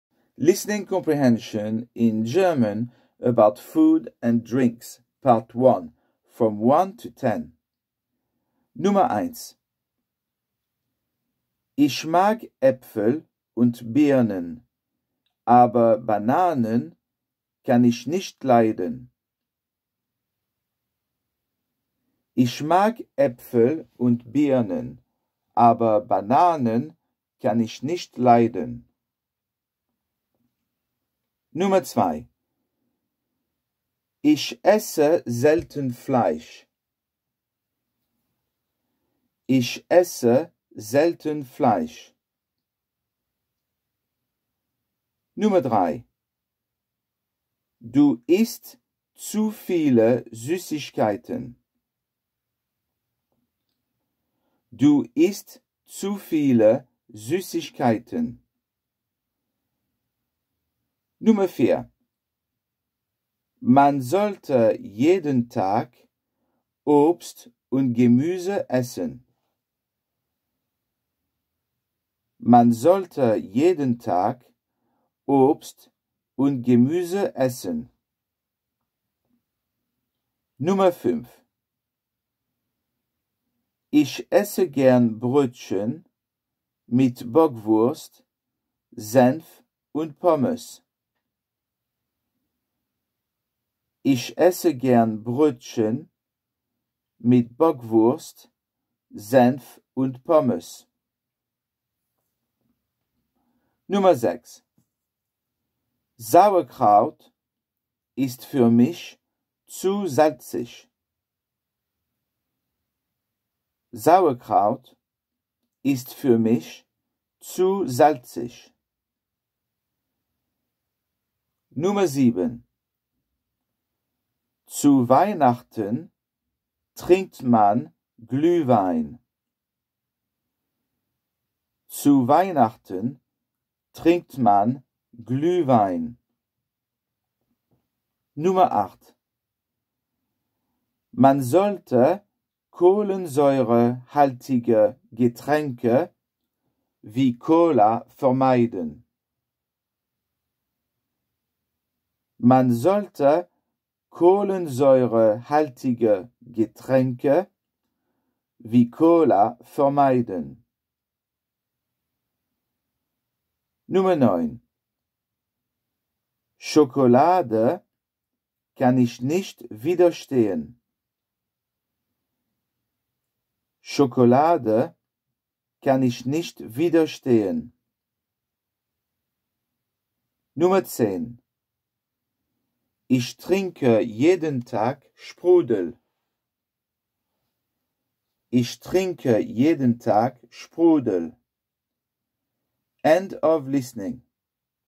FOOD AND DRINKS (GERMAN LISTENING)
1. Ich mag Äpfel und Birnen aber Bananen kann ich nicht leiden.

2. Ich esse selten Fleisch.
3. Du ißt zu viele Süßigkeiten!
4. Man sollte jeden Tag Obst und Gemüse essen.
5. Ich esse gern Brötchen mit Bockwurst, Senf und Pommes.
6. Sauerkraut ist für mich zu salzig.
7. Zu Weihnachten trinkt man Glühwein.
8. Man sollte Kohlensäurehaltige Getränke wie Cola vermeiden.
9. Schokolade kann ich nicht widerstehen.
10. Ich trinke jeden Tag Sprudel.